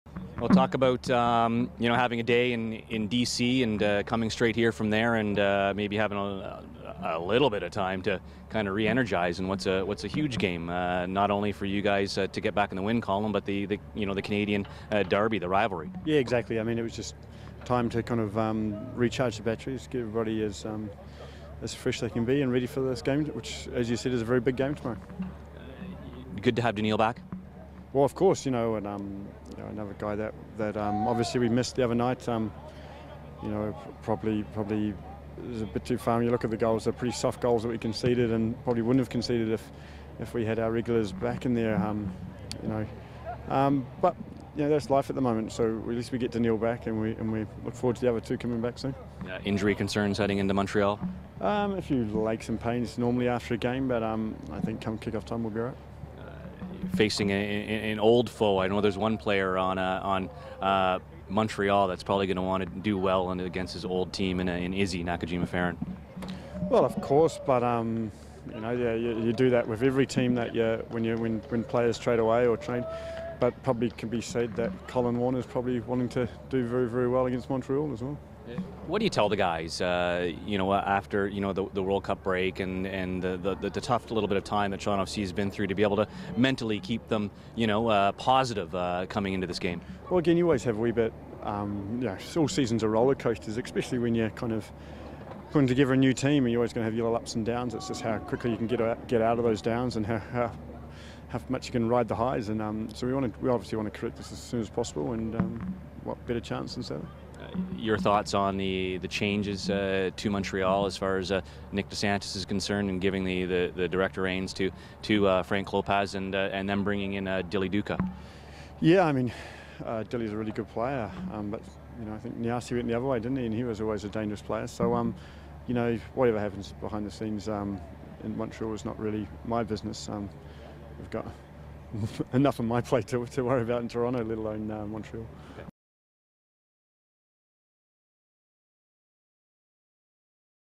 Ryan Nelsen - August 1, 2014
Ryan Nelsen speaks with media ahead of tomorrow's match against Montreal.